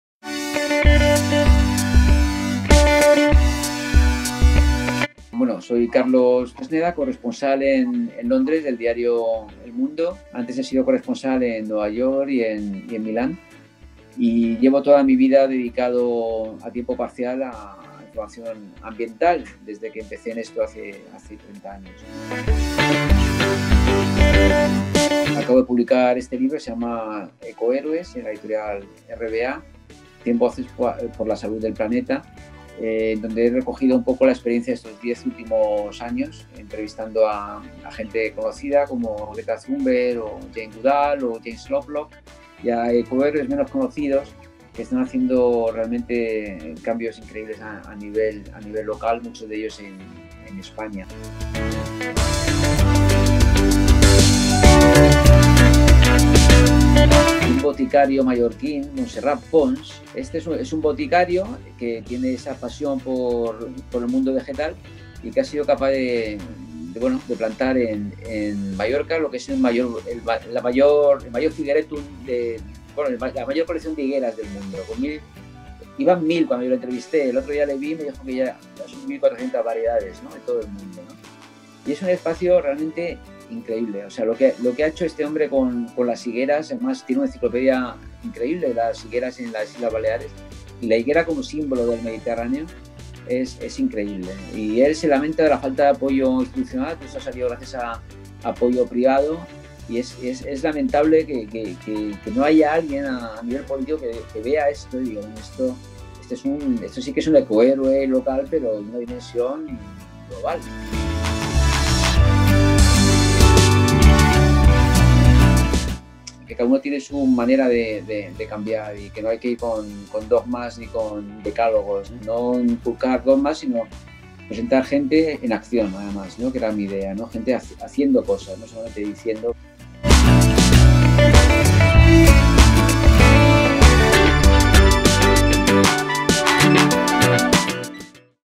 Carlos Fresneda: “Sería conveniente quitarle ideología a lo verde” | Descubriendo a...
El periodista climático, que no tiene coche desde hace treinta años, cree que “la lucha contra el cambio climático no debería ser de izquierdas o de derechas”. En su libro, cuenta la historia de cien ‘ecohéroes’ que dedican su vida a luchar por el bienestar del planeta.